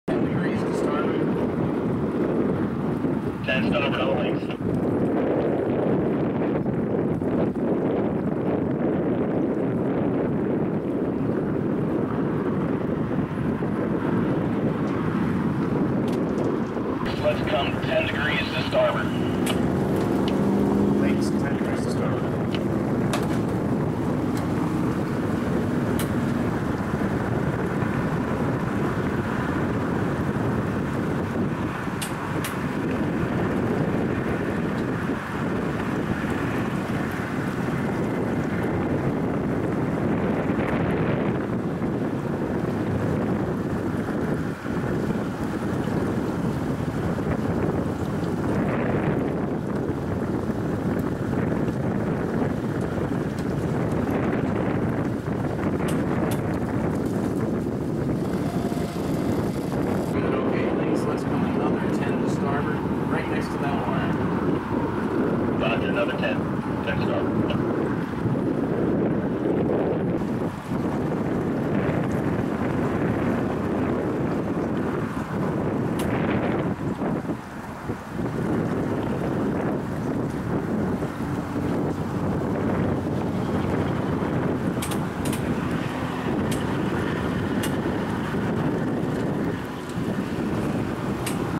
Dutton's Navigation - Rig To Tug Boat Commands
Dutton's Navigation Rig Moving School for Jack ups allows you to experience what it takes to be on both ends of the radio during the rig move.  As a student you will have time both on the jack up rig as the rig mover and on the tug boat as the boat captain.  As the boat captain you will gain an better appreciation and understanding of what it takes to move a jack up drilling rig.